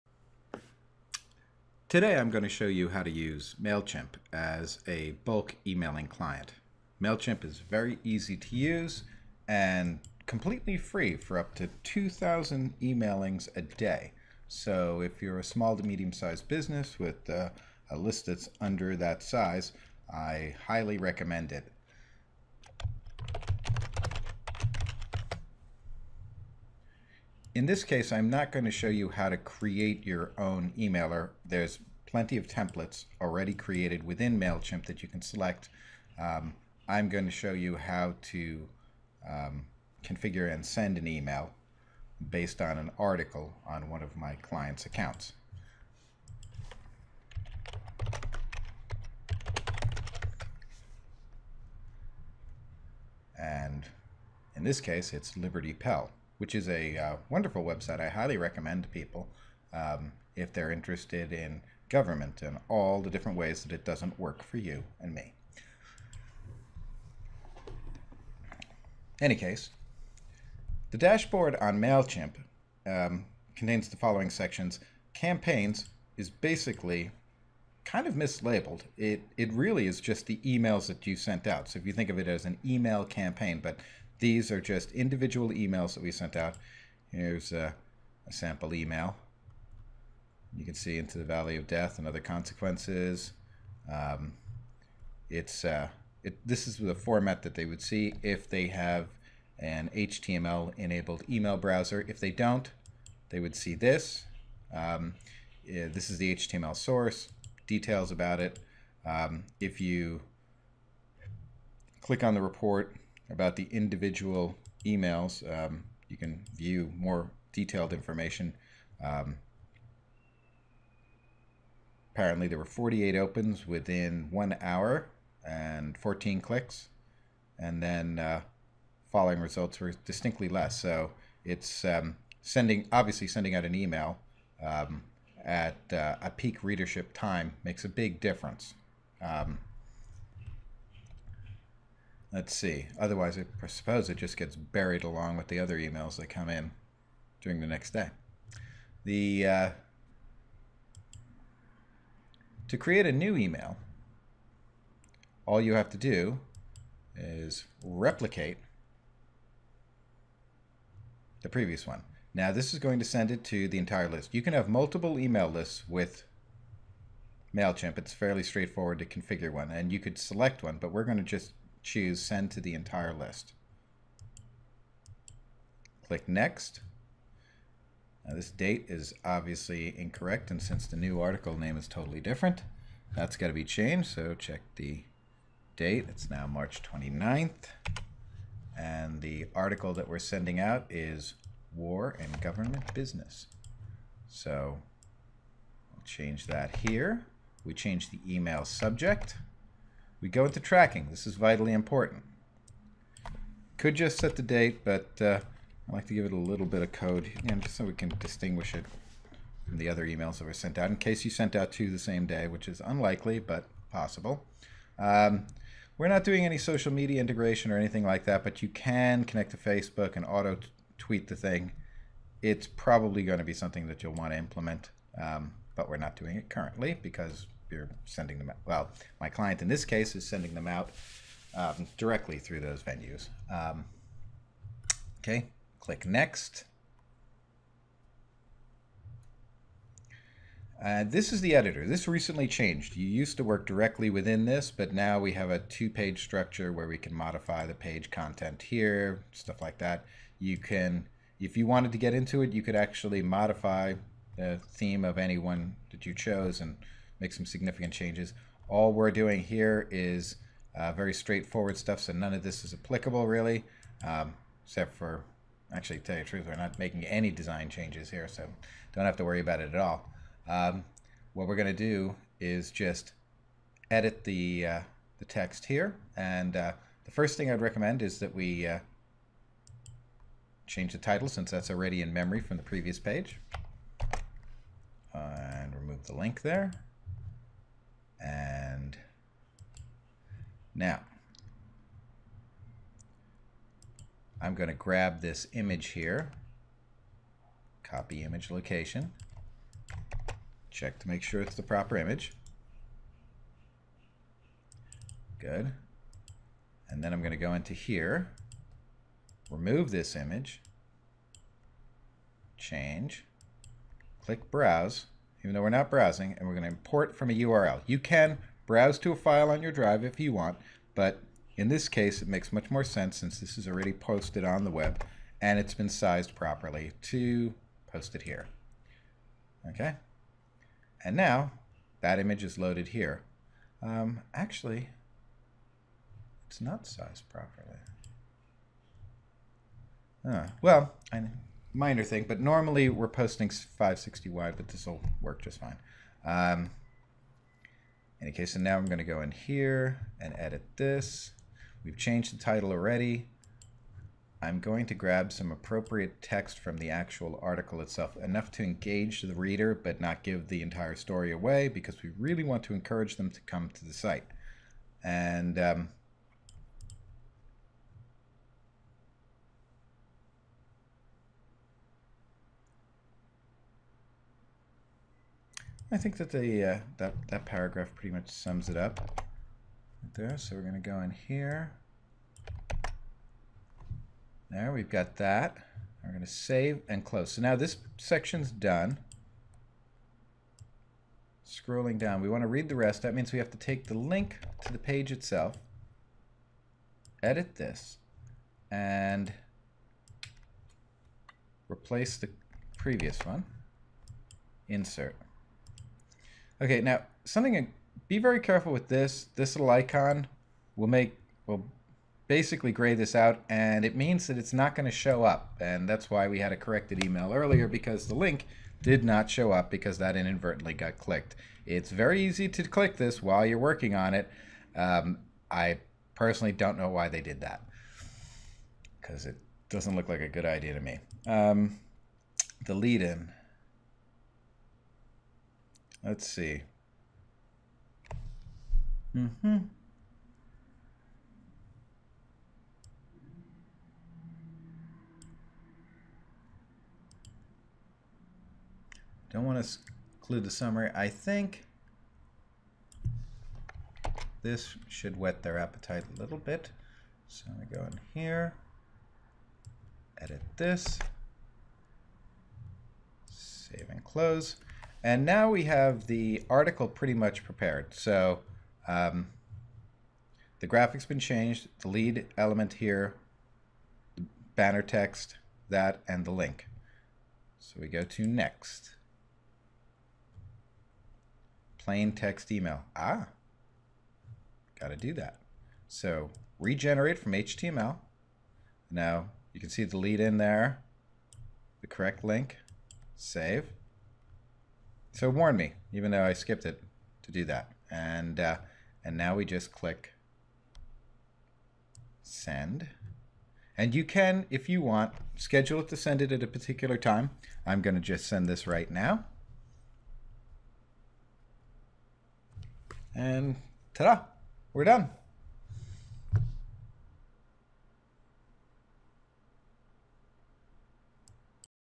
MailChimp Emailer Tutorial
A simple introduction into MailChimp with step by step instructions for sending an emailer to your subscriber list.